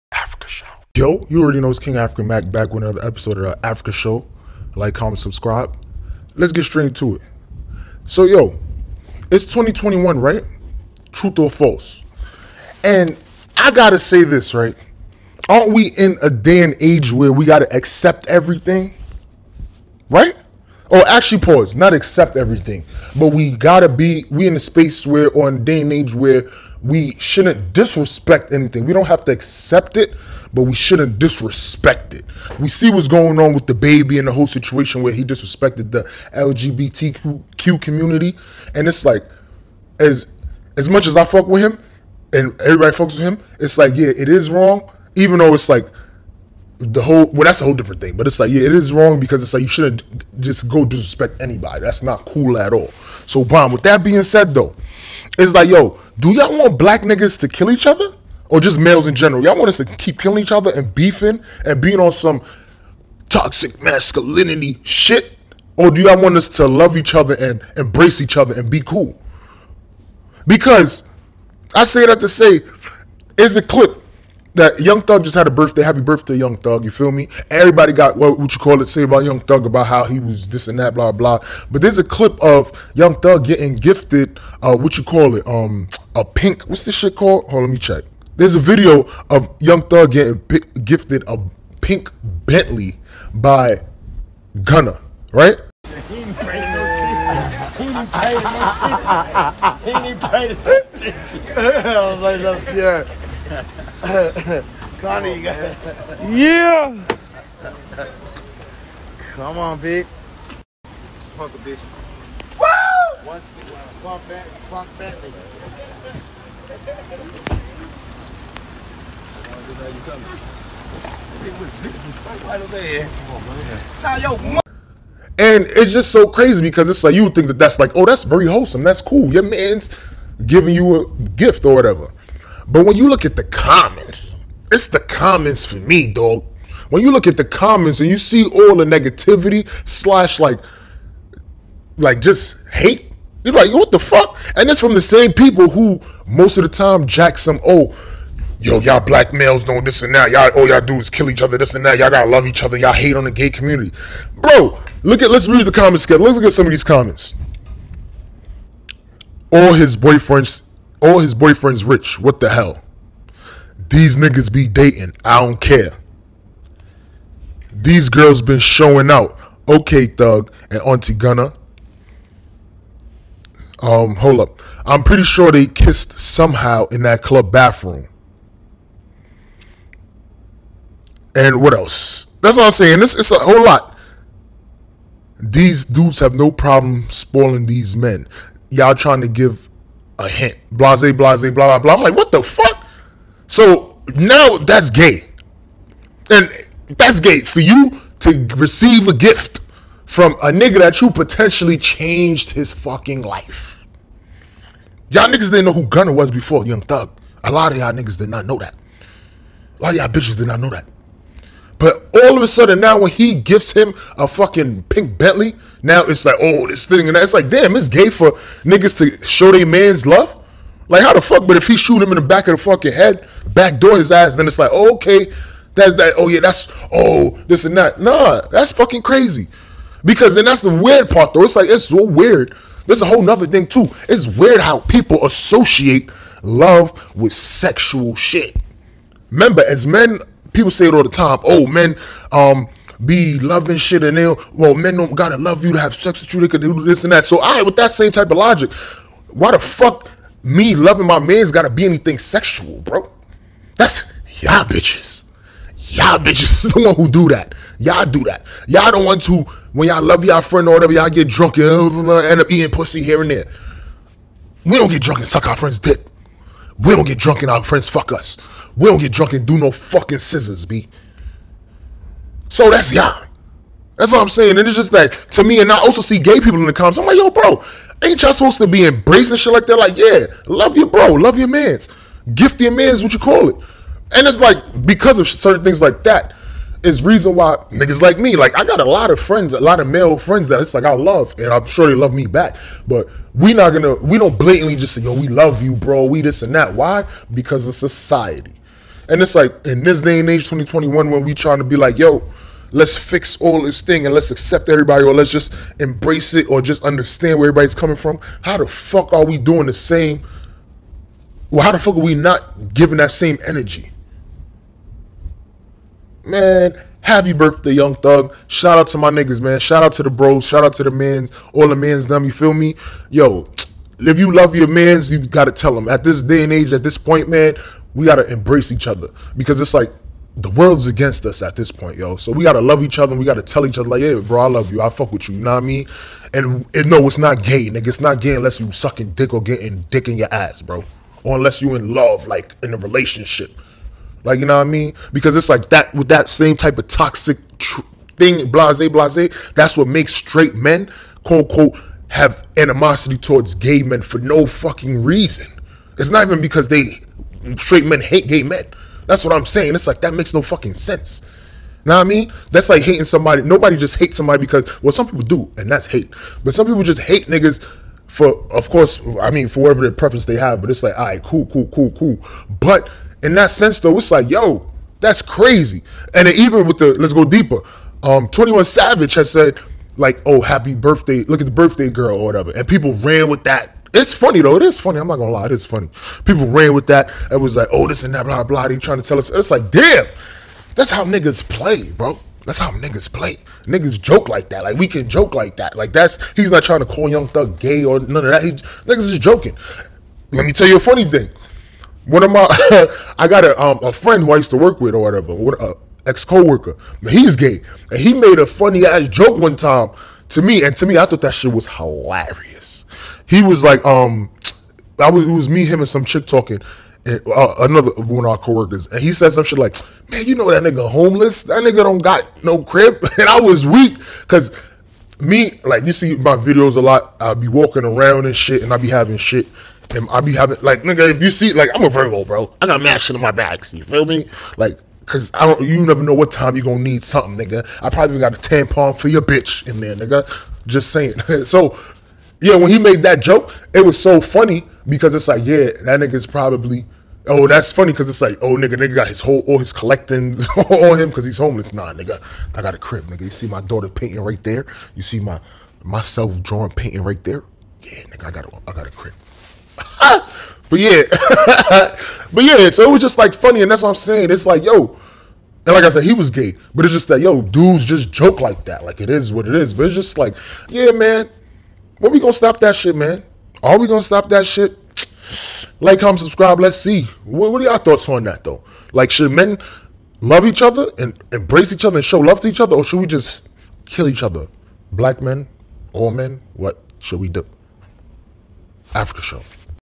Is it Gay for Men to Gift their friends gifts?
Gunna gifted Young Thug a "Pink Bentley" for his bday and people are calling it Gay! Do you agree or disagree?

Comment down Below1